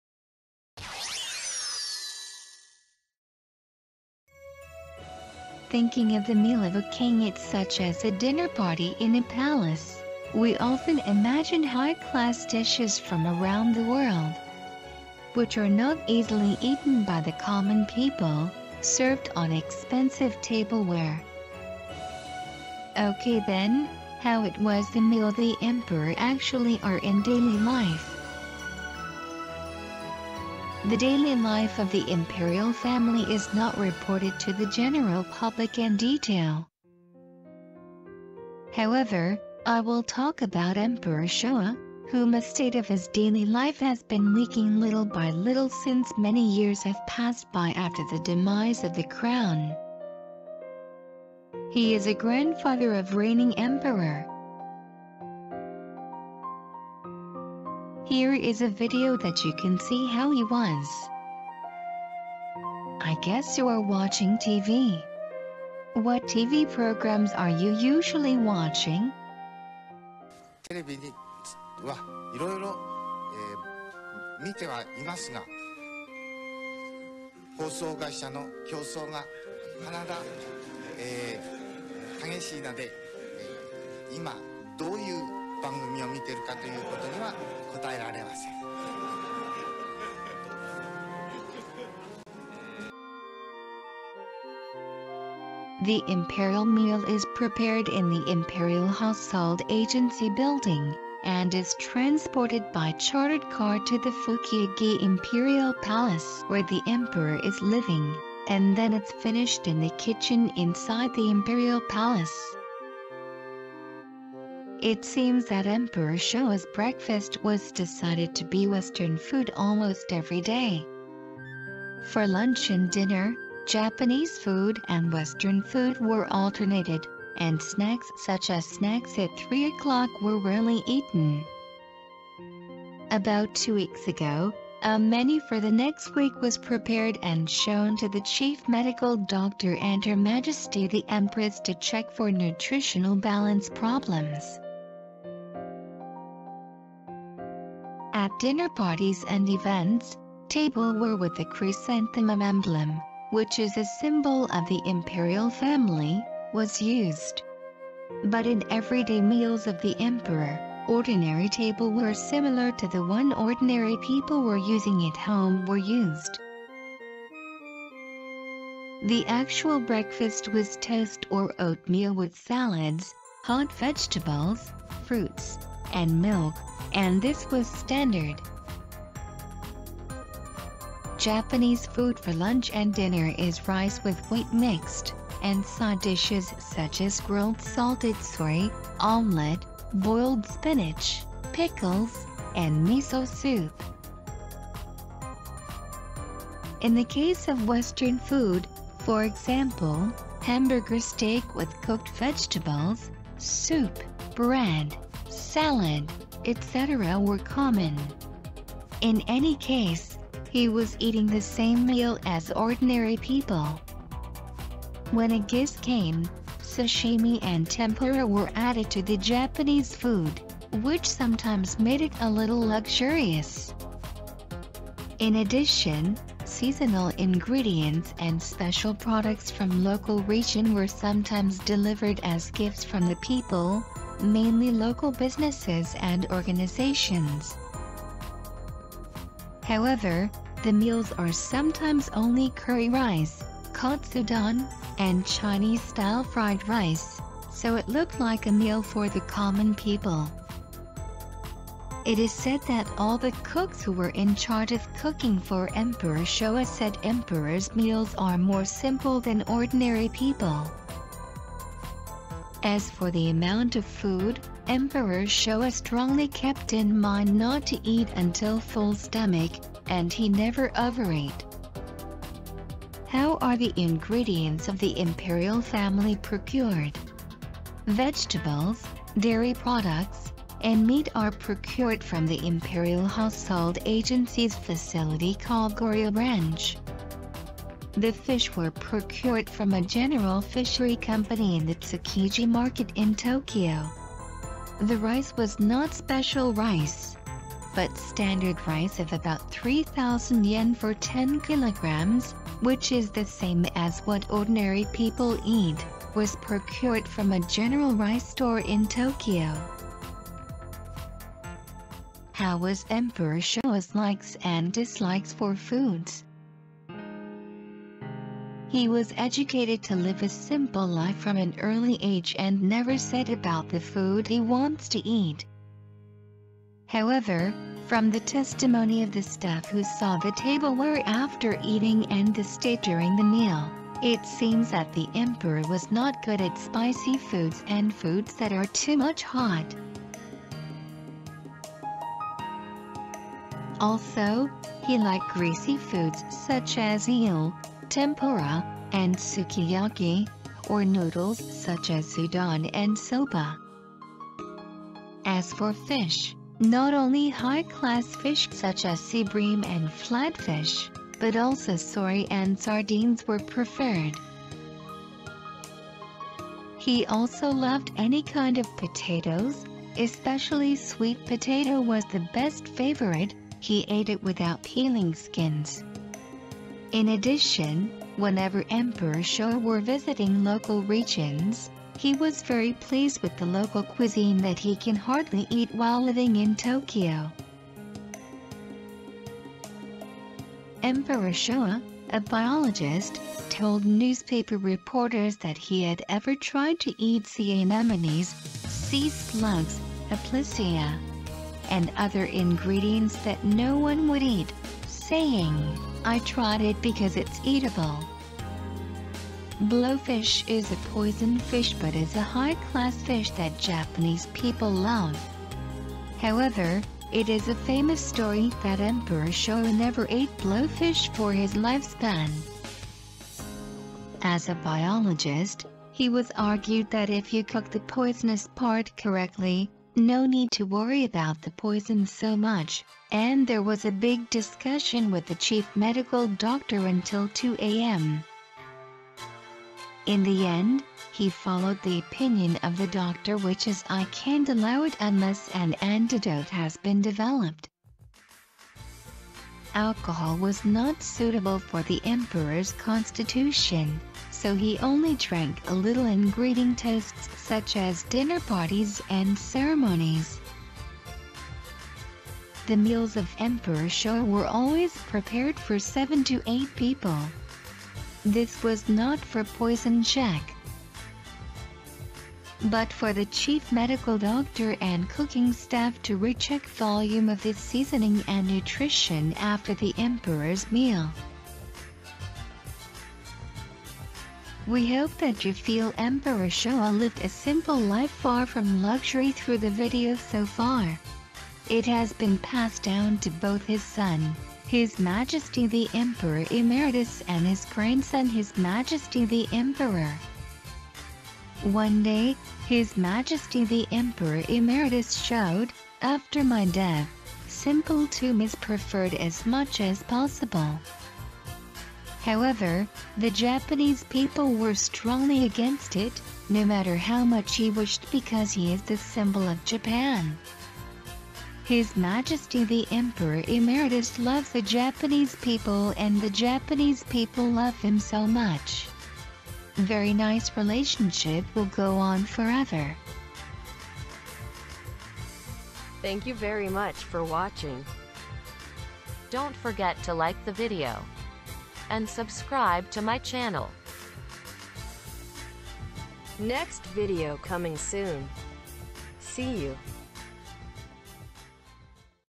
The Japanese Emperor’s Meal | What Japanese Emperor is eating in daily life
The daily life of the imperial family is not reported to the general public in detail. However, I will talk about Emperor Showa, whom a state of his daily life has been leaking little by little since many years have passed by after the demise of the crown.
He is a grandfather of Reigning Emperor.

daily life